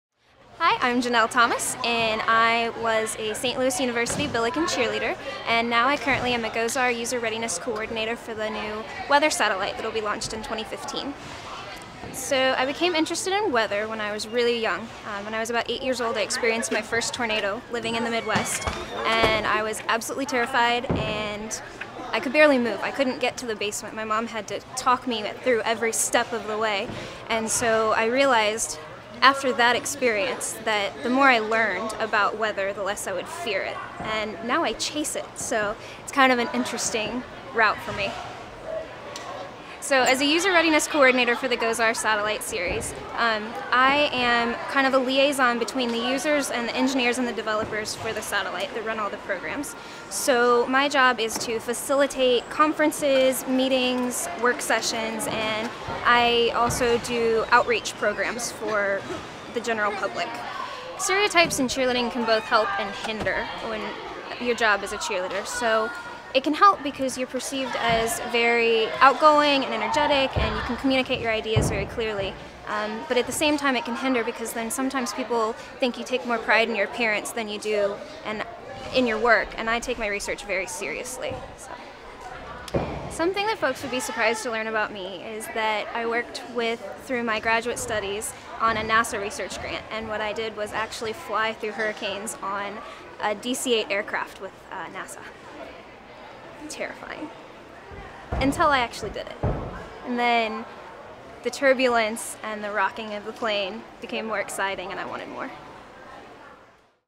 Science Cheerleader
Current and former NFL and NBA cheerleaders pursuing science, technology, engineering and math careers. Playfully challenging stereotypes, inspiring young women towards science careers, engaging people from all walks of life in citizen science projects